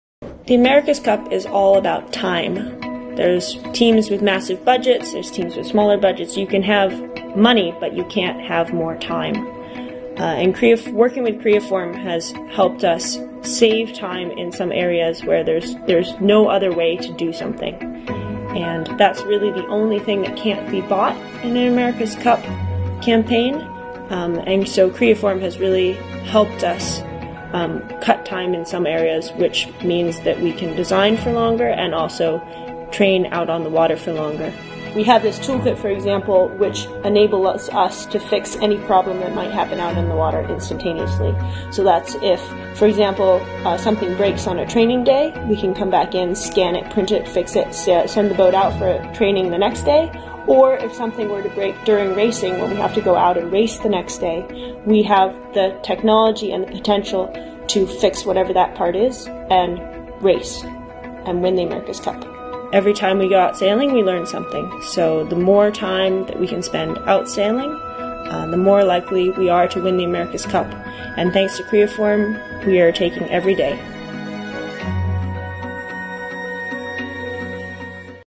New York Yacht Club’s American Magic uses Stratasys 450mc to 3D printed sailing yacht parts
The race is on…even before the 2021 America’s Cup race in March 2021! Discover how Robyn Lesh and other members of the design team for New York Yacht Club’s American Magic actually save hundreds of man-hours in developing their monohull, the PATRIOT, and actually gain more training time!